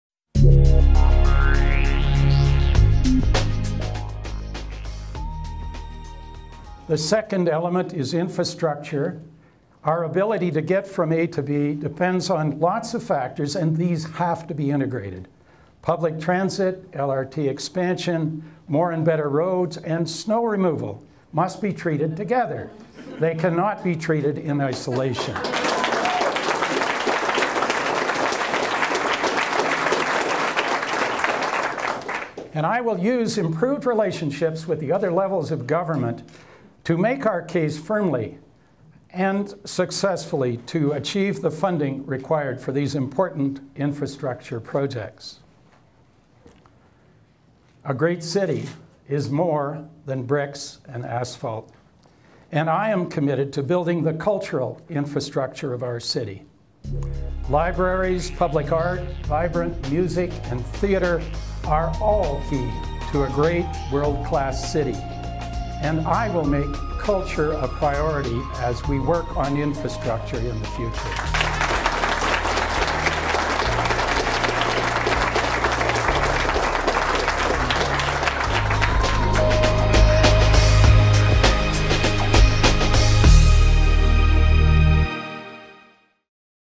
Wayne Stewart on Calgary Infrastructure
Listen to Wayne talk about integrating public transit, LRT expansion, road expansion and maintenance, and snow removal. Cultural Infrastructure is also very important to us.